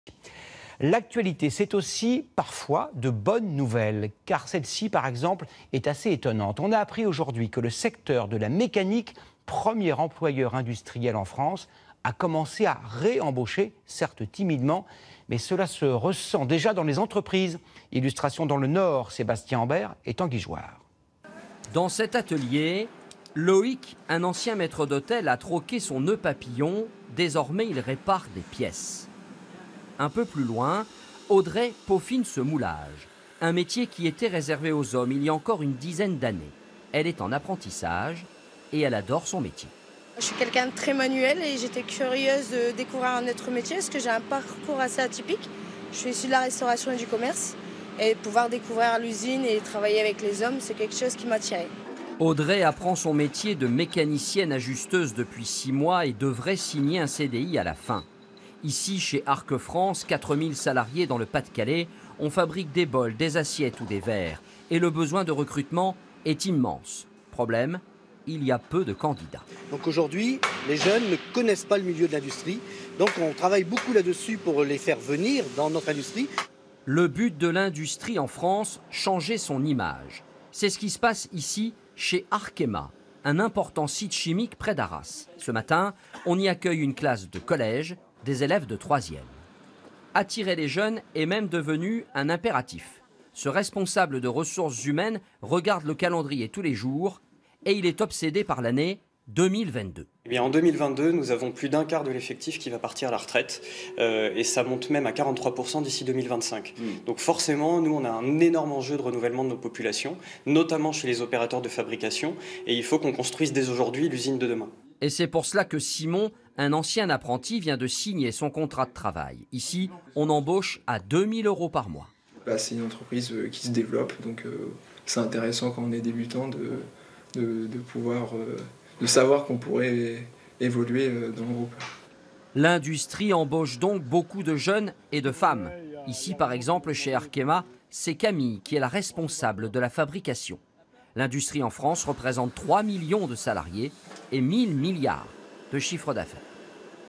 [TF1] L'industrie veut attirer les jeunes et les femmes
Reportage de TF1 sur la dynamique de recrutement dans l'industrie et chez Arkema, dans notre usine de Feuchy dans le Nord où des ateliers de sensibilisation aux jeunes publics sont organisés. Objectif : changer l'image de l'industrie et attirer les talents.